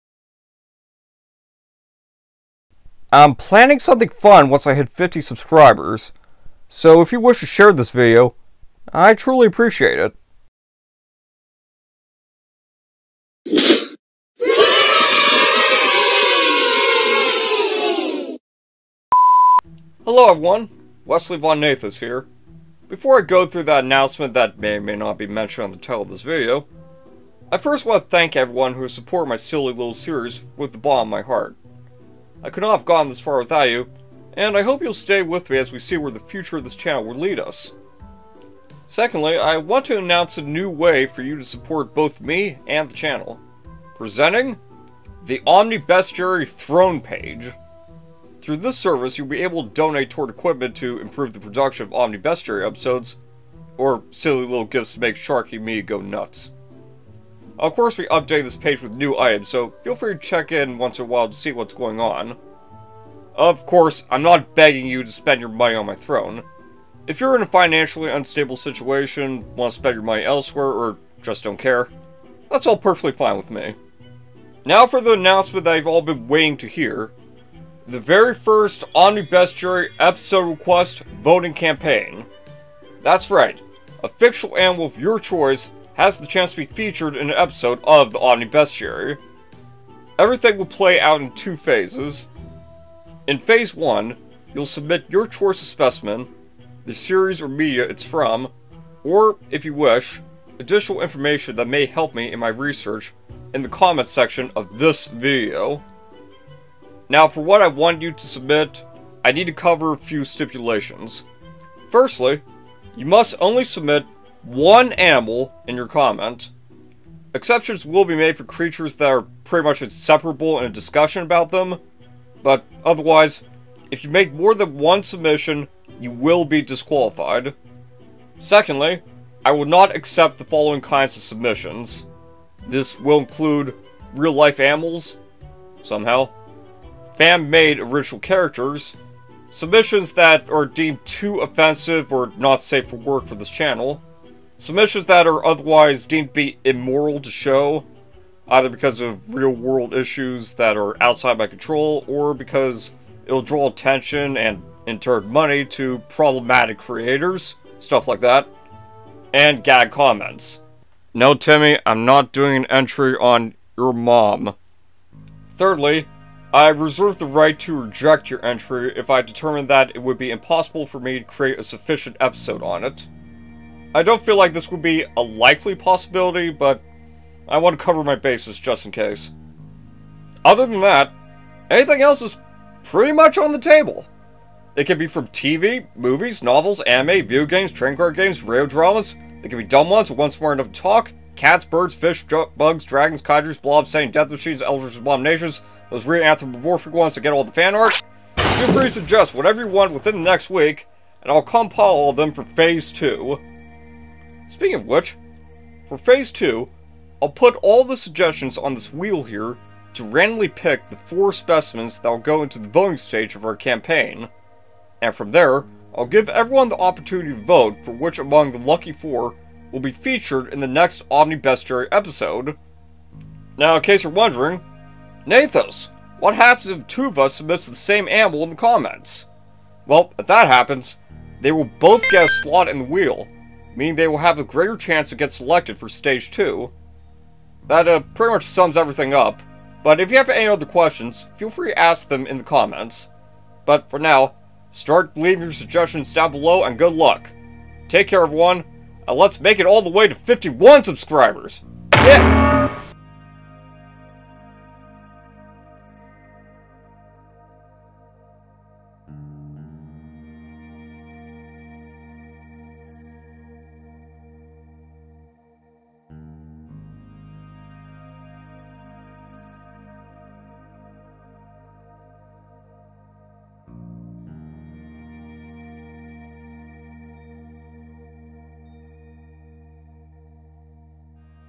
50 Subscriber Celebration! Episode Request Campaign! Other Exclamations!
Another Rundown of the Episode Request Campaign:
Phase 1:
To participate, submit only ONE fictional animal, the series or media it's from, and (if you want) additional information to help me with my research in the COMMENTS SECTION of THIS VIDEO.
Exceptions can be made for multiple animals that are inherently linked with each other.

DO NOT submit:
Real Animals
Fan-made OC's
Offensive Content
Overly NSFW Content
Content that is determined to be problematic or immoral (i.e. real world issues, problematic creators, etc.)
Gag Comments

FAILURE TO FOLLOW THESE RULES MAY RESULT IN DISQUALIFICATION

I reserve the right to reject your submission, if I determine that it would be otherwise impossible for me to create a sufficient episode for it.

Phase 2:
Four submissions from Phase 1 will be randomly selected. You can vote for these four (via YouTube's community tab), and the winner will have an episode of the Omni-Bestiary created for them.